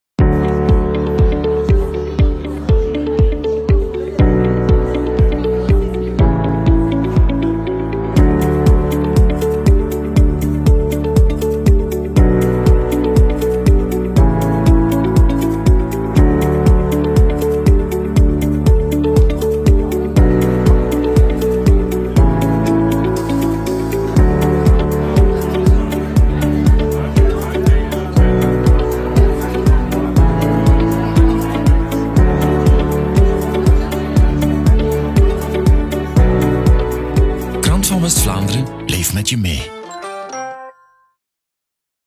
Tentoonstelling Spijker en Schardauw
De vernieuwingsoperatie voor de wijk Spijker en Schardauw in Kuurne krijgt stilaan vorm. Dinsdagavond werd in het buurthuis op de wijk de voorstudie van het Gentse architectenbureau Stéphane Beel Architects voorgesteld aan de buurtbewoners.